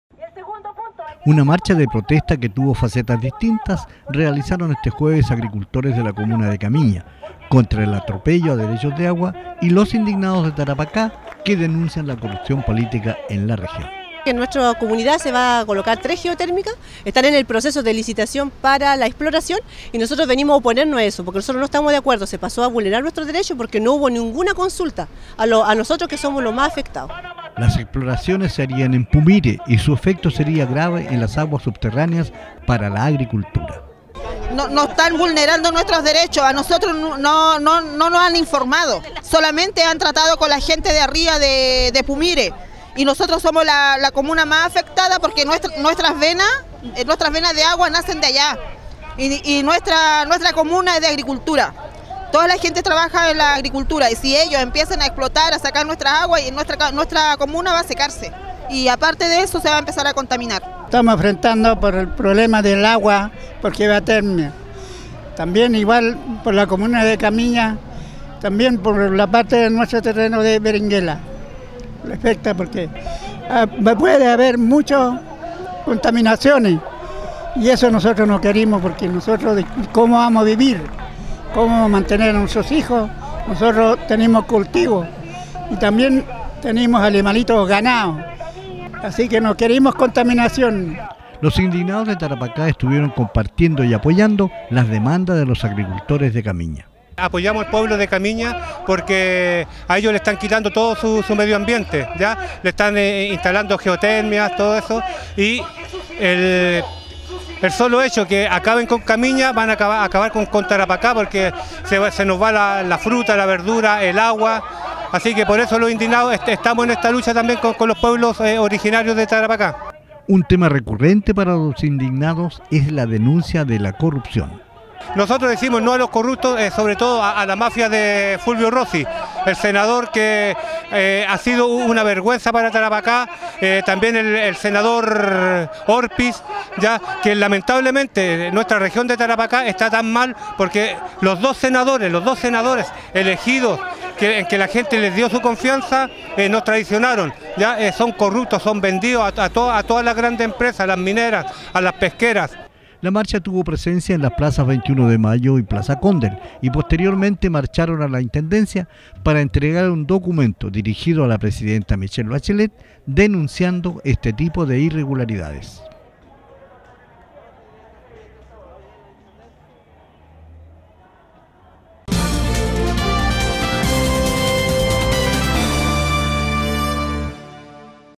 DERECHOS DE AGUA Y CORRUPCIÓN MOTIVÓ MARCHA CIUDADANA - Iquique TV
(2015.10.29) Una marcha de protesta, contra las mineras y geotérmicas y contra la corrupción política en la región, realizaron este jueves integrantes de la comuna de Camiña y un grupo de los Indignados de Tarapacá.
Los camiñanos  alegan pérdida de derechos y rechazan el inicio de estudios, para el uso industrial del agua, por temor a la contaminación y la pérdida de sus terrenos de cultivo, y los indignados denuncian que la corrupción ha privado a la región de la representación senatorial.